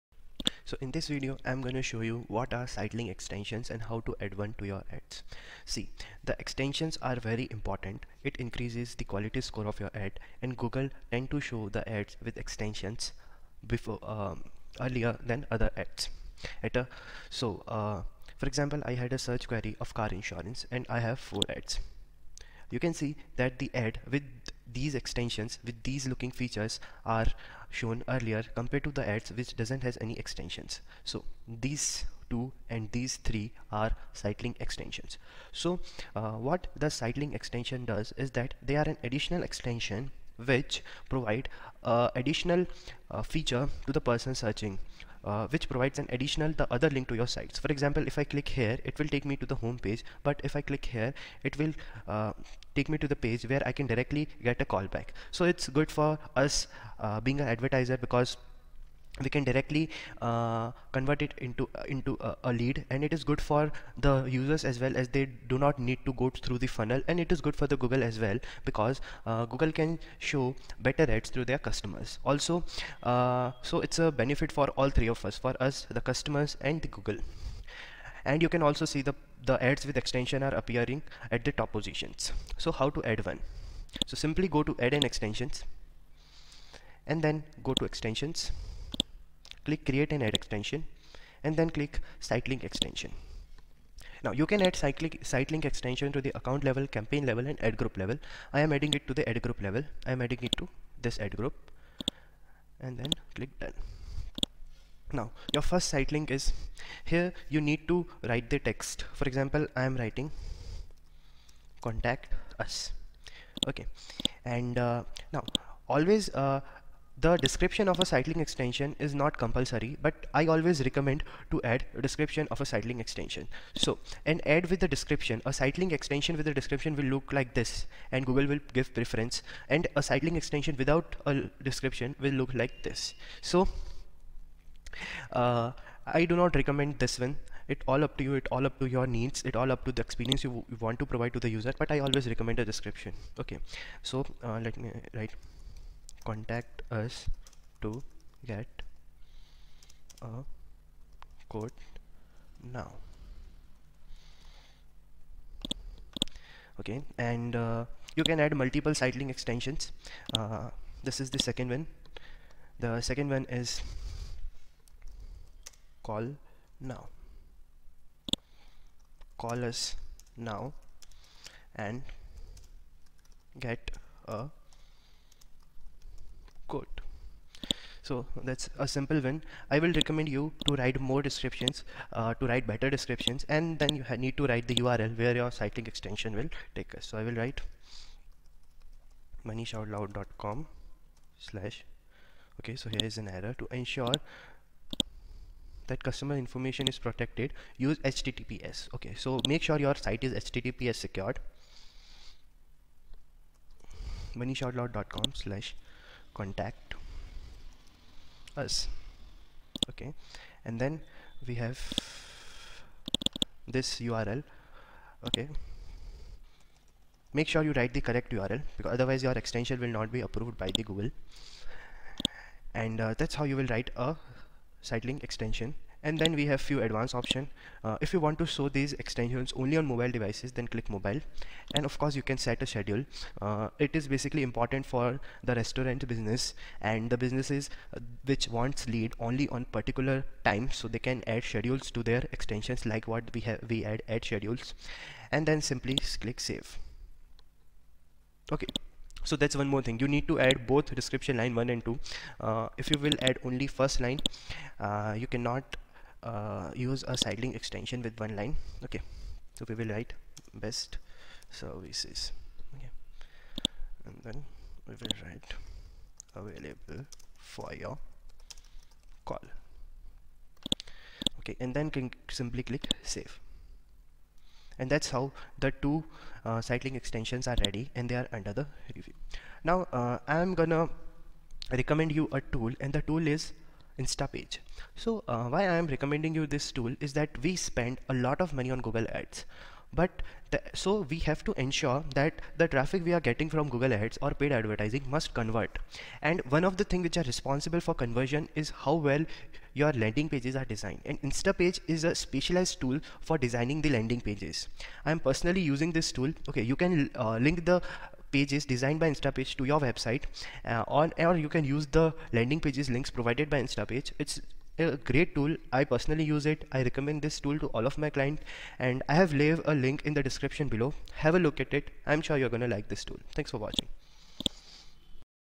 Site Link Extension in Google Ads (Adwords)
To add more links to your ads, create sitelink extensions. Sitelinks can take people to specific pages on your site—your store hours, a specific product, or more. When someone clicks or taps on your links, they skip right to what they want to know or buy.